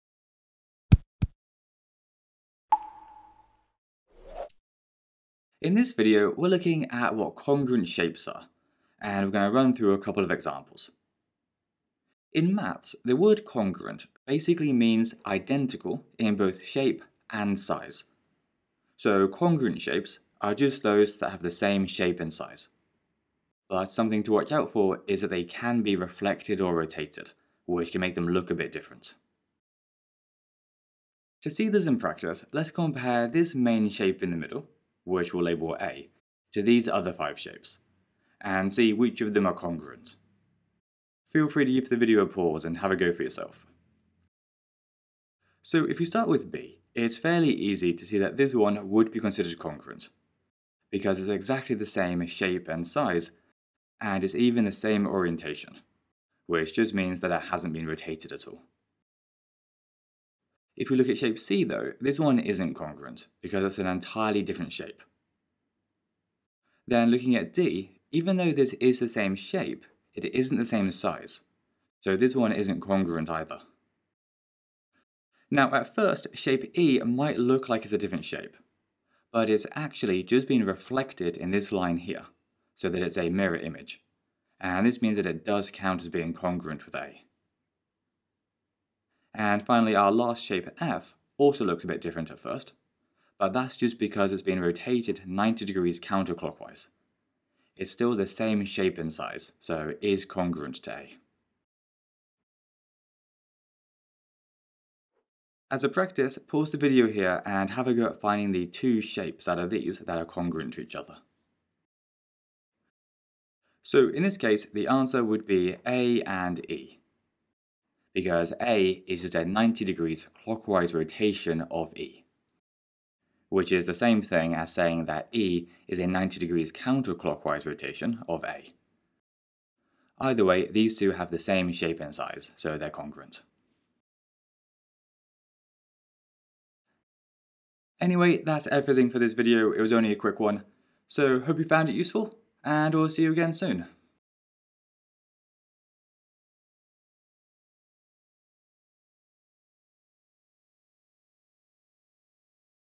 GCSE Maths - Congruent Shapes (2026/27 exams)
*** WHAT'S COVERED ***
1. Definition of Congruent Shapes.
2. Identifying Congruent Shapes.
3. Worked Examples:
    * Identifying shapes congruent to a given shape.
    * Finding pairs of congruent shapes from a selection.

A-level - Not in your course

*** CHAPTERS ***
0:00 Intro & Definition
0:31 Example 1: Identifying Congruent Shapes
1:44 Example 2: Practice Identifying Congruent Shapes